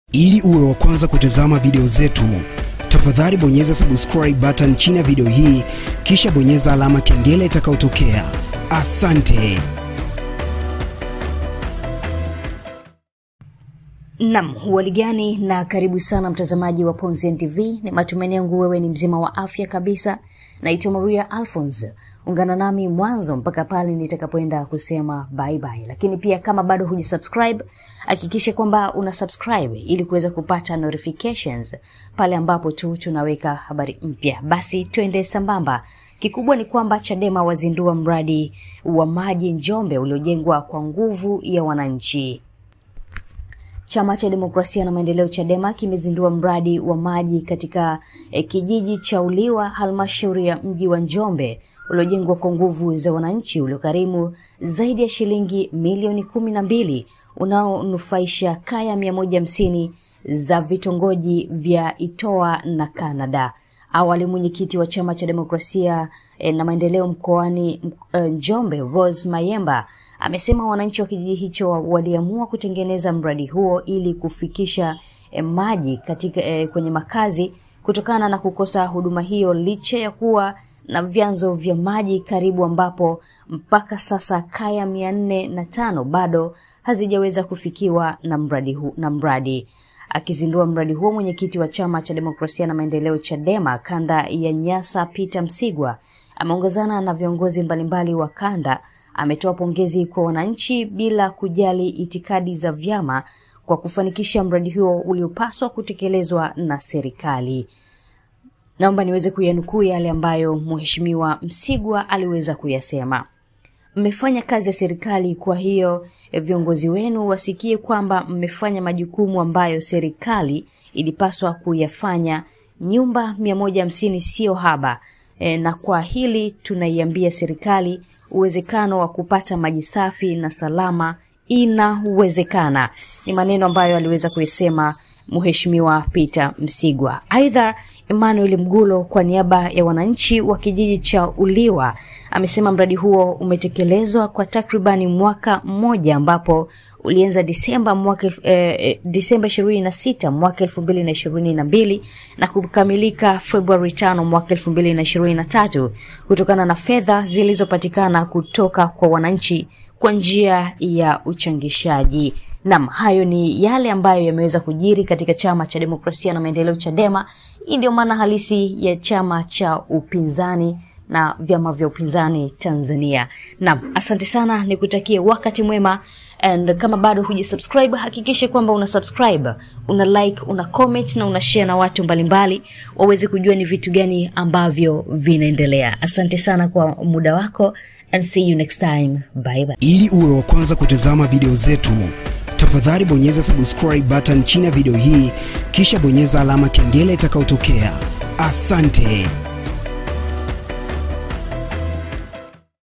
Video hi Inamfikia Rais Samia; Chadema wafanya tukio hili huko Njombe, Mchana kweupe
CHADEMA wazindua mradi wa maji Njombe uliojengwa kwa nguvu ya wananchi